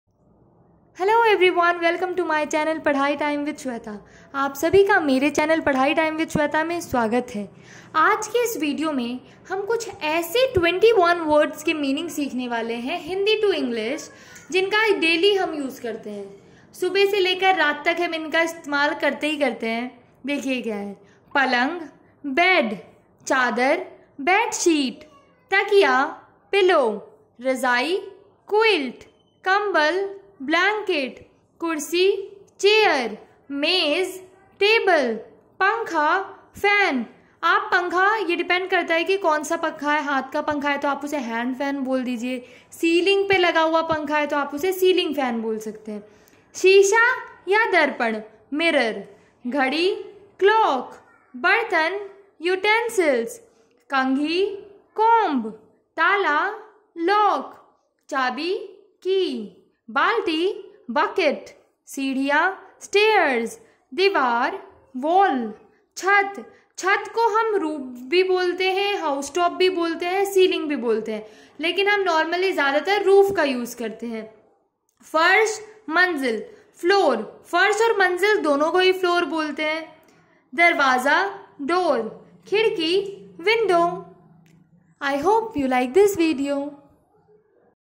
21 word meaning (household items) Hindi to English/ English speaking practice
In this video you will learn 21 basic and important word meaning (household items) Hindi to English.which we use daily in our life.